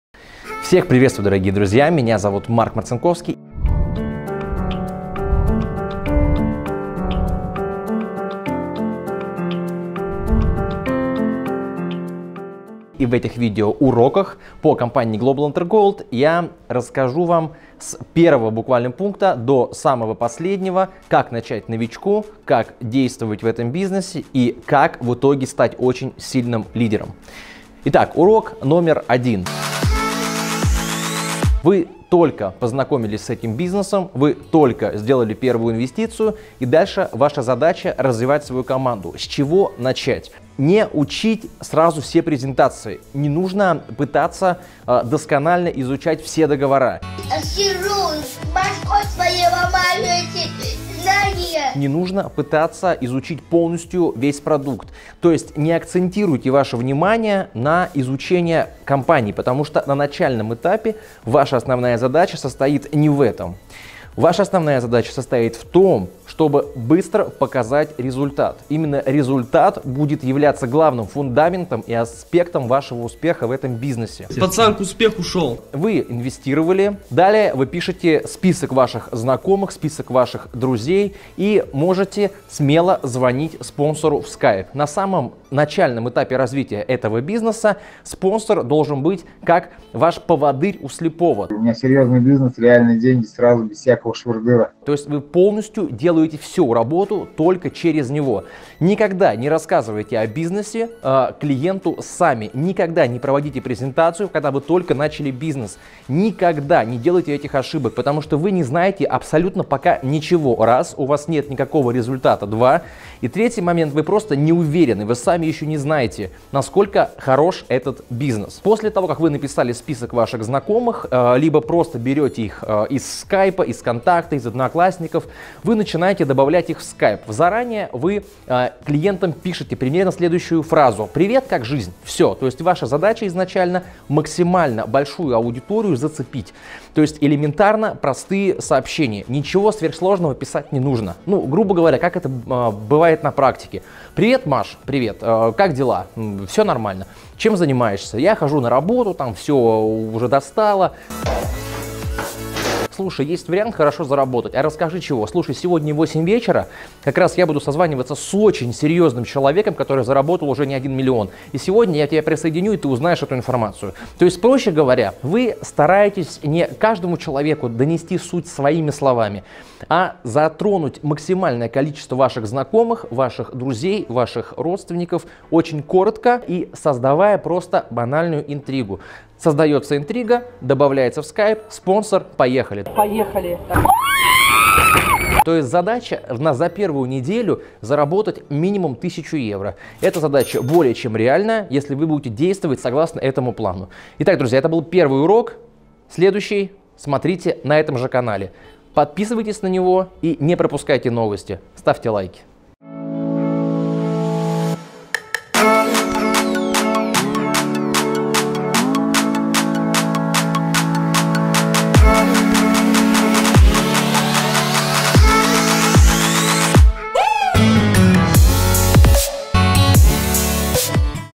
ШКОЛА GLOBAL İNTERGOLD. БИЗНЕС- УРОК 1. С ЧЕГО НАЧАТЬ БИЗНЕС НОВИЧКУ?
БЕСПЛАТНО! НАЧИНАЕМ БИЗНЕС- УРОКИ.
ВСЕГО 7 БИЗНЕС-УРОКОВ. ОБЯЗАТЕЛЬНО ВСЕ ПОСМОТРИТЕ! ОНИ ВАС МНОГОМУ НАУЧАТ!
С ЧЕГО НАЧАТЬ НОВИЧКУ  ?
КАК НАУЧИТСЯ ПРИГЛАШАТЬ ЛЮДЕЙ?
КАК ПИСАТЬ СООБЩЕНИЯ?
КАК ПРЕДЛОЖИТЬ БИЗНЕС?
МАРК МАРЦИНКОВСКИЙ---ТОП-ЛИДЕР ПОДРОБНО ВСЕ РАССКАЖЕТ И НАУЧИТ ВАС..